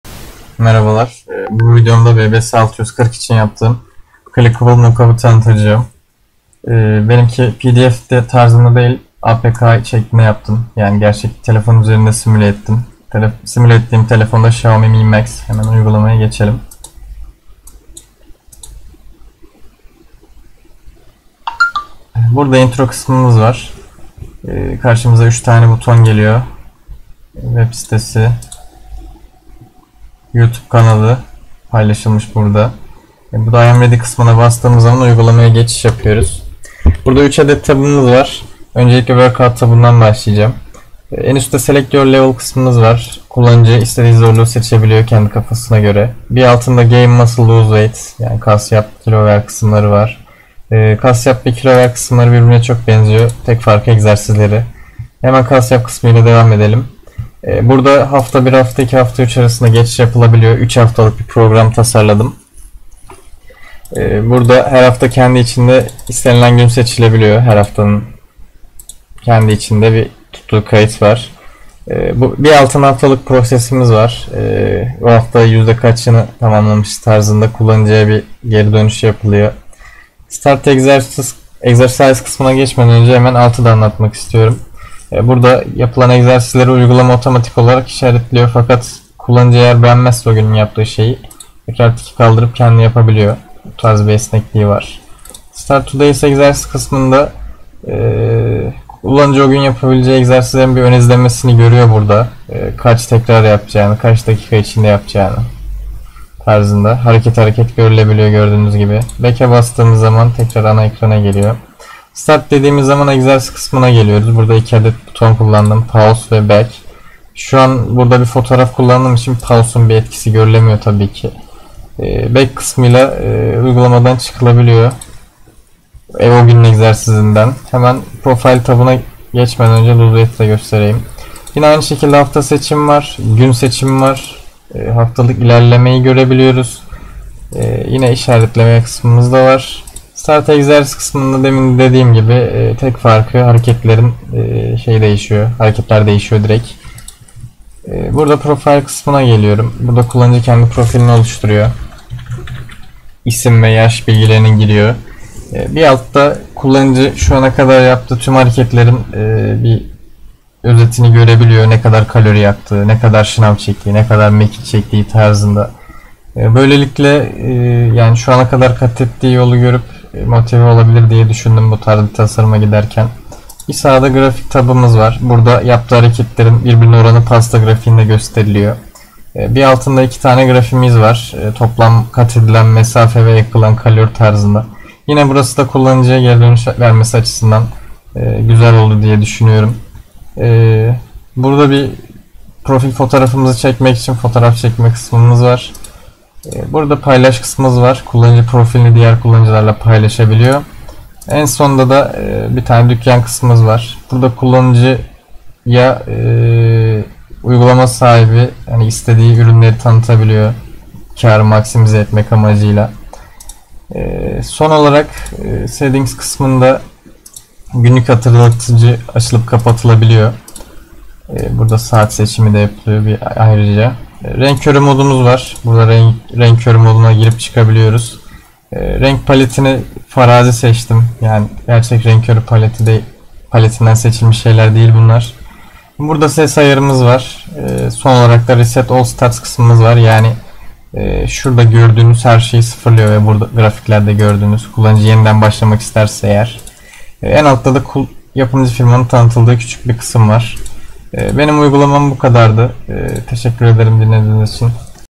Fitness Application for Mobile Devices - Unity Mobile Application Demo
This is a demo application for Mobile Platforms. It is built in Unity.

Unity Android Application,
Unity fitness application,
Unity mobile application demo,